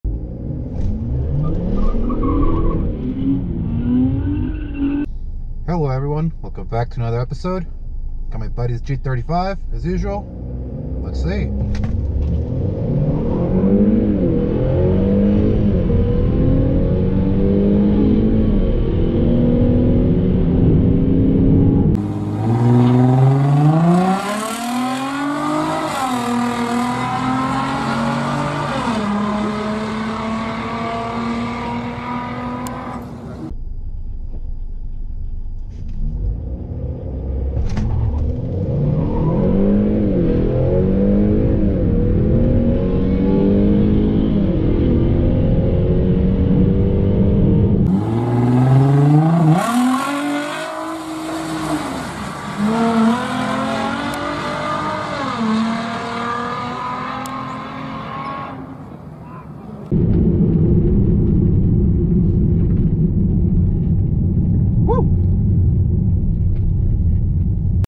2022 Honda Accord Sport 2.0T vs. 2007 Infiniti G35
Stock Accord 2.0T (10-Speed) vs. Bolt-on G35